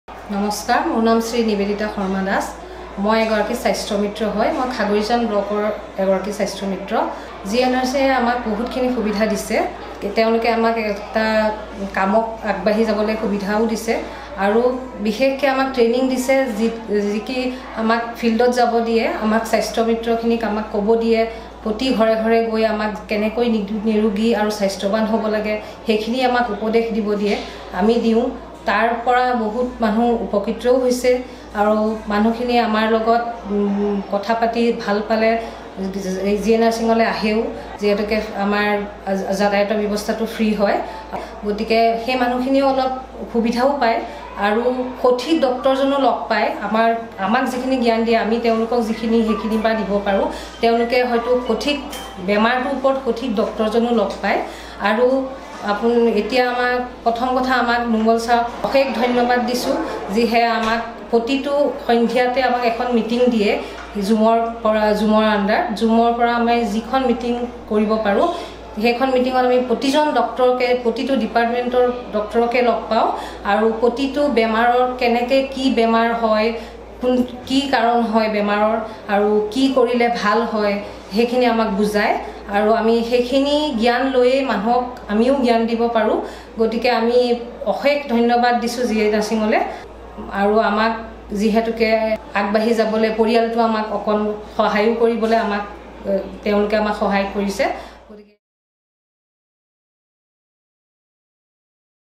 SwasthyaMitra | Ms Nivedita Sharma Das
Ms Nivedita Sharma Das is a SwasthyaMitra from Nagaon. We are proud of her for her sincere efforts in bringing quality and affordable healthcare to the masses.